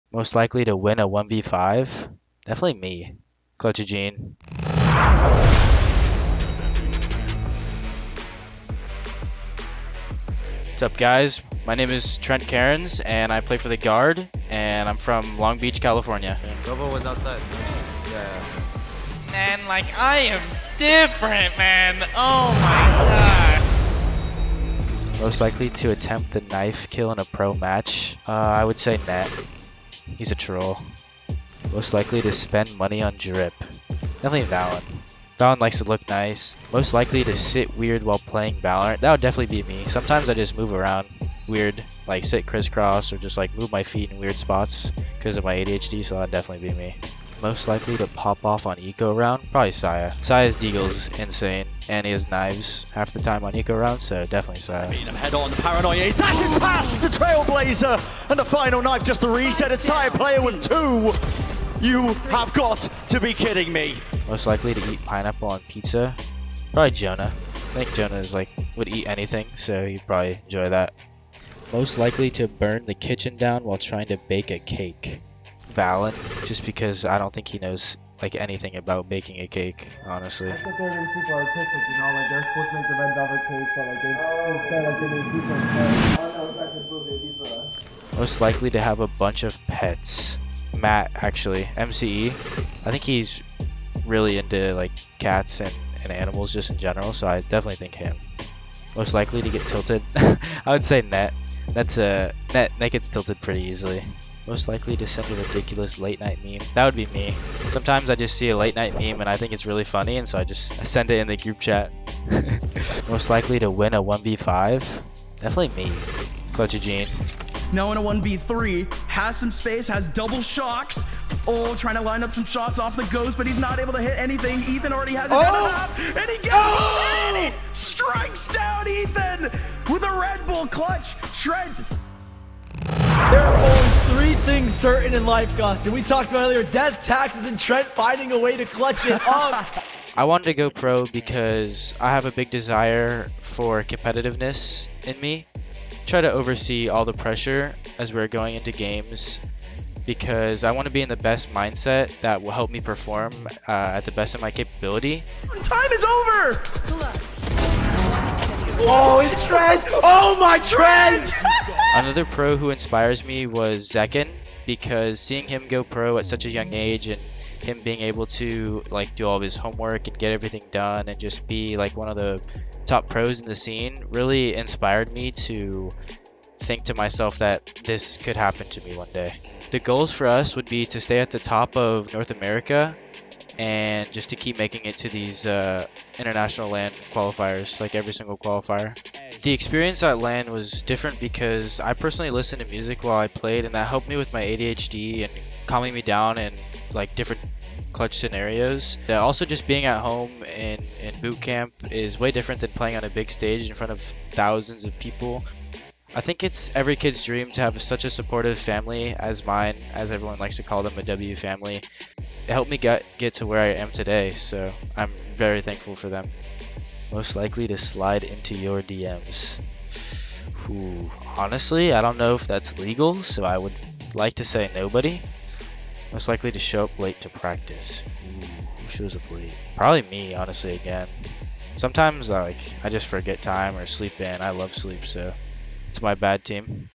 Trent, da The Guard, joga "Quem Você Acha" | Jogo Rápido
Colocamos o Trent da The Guard nos holofotes para jogar "Quem Você Acha". Ele também falou sobre seus objetivos pessoais para o restante da Temporada do VCT 2022 e revelou qual profissional é sua maior inspiração.

Acompanhe nossos canais para ficar por dentro de todas as notícias e as melhores jogadas do VALORANT Champions Tour: